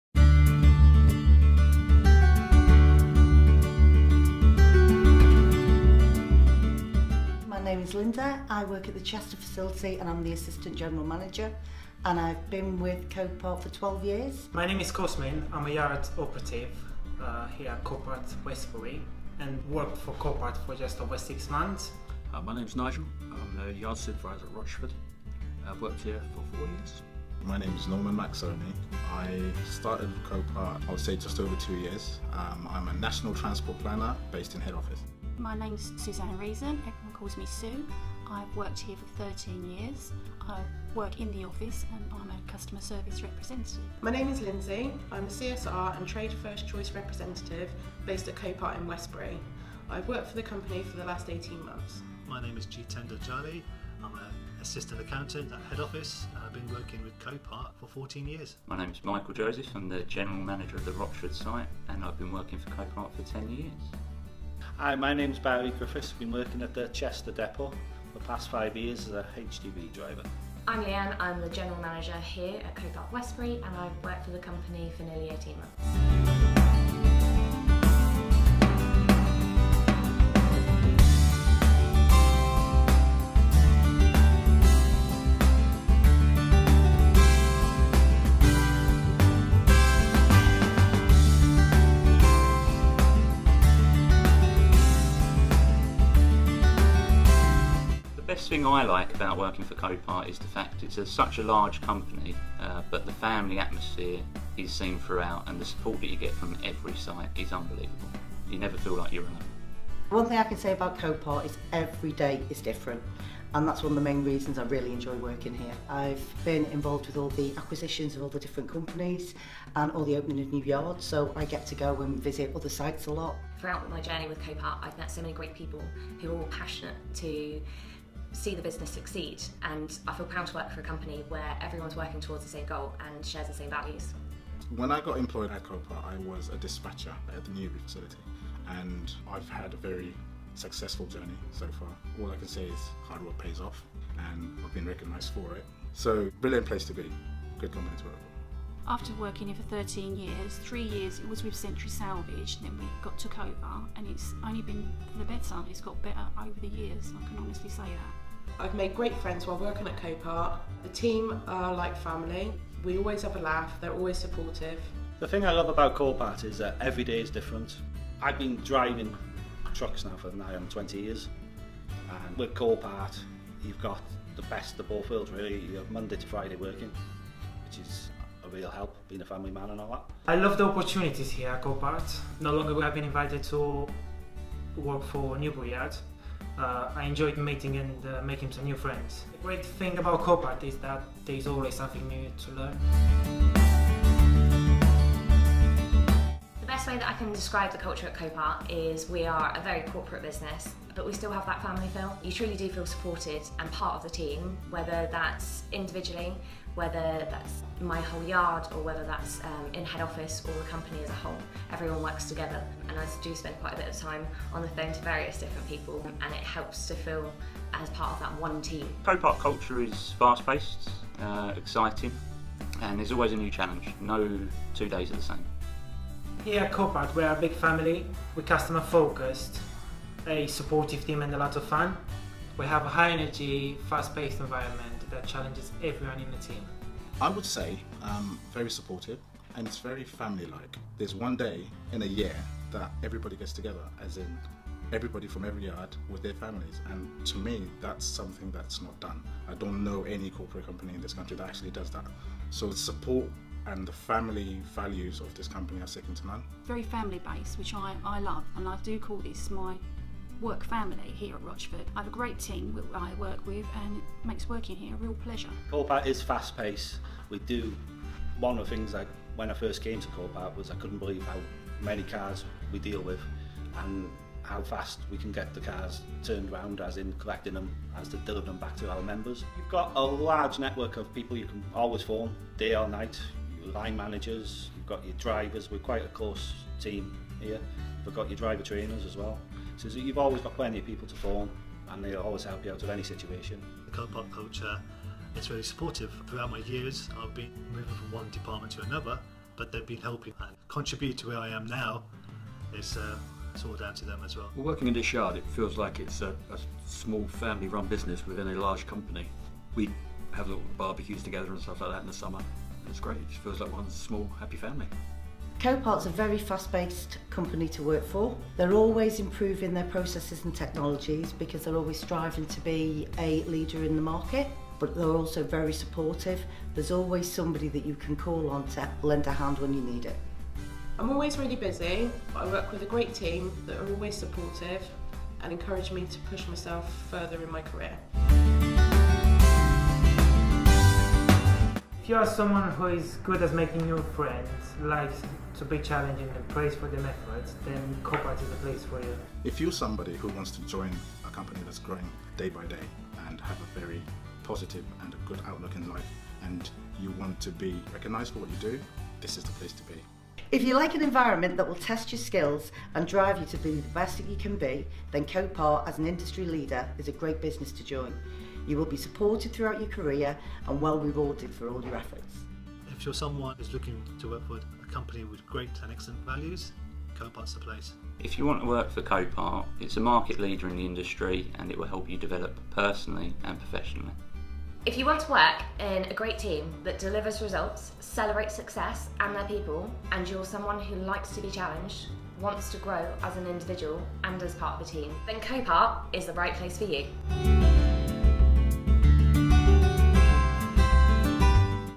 Careers with Copart UK Limited
At Copart UK Limited, we continuously strive to make ourselves a company that our colleagues and their families are proud to be a part of. Watch our video to hear directly from our colleagues about the different job roles we have to offer and what it’s like to work for Copart.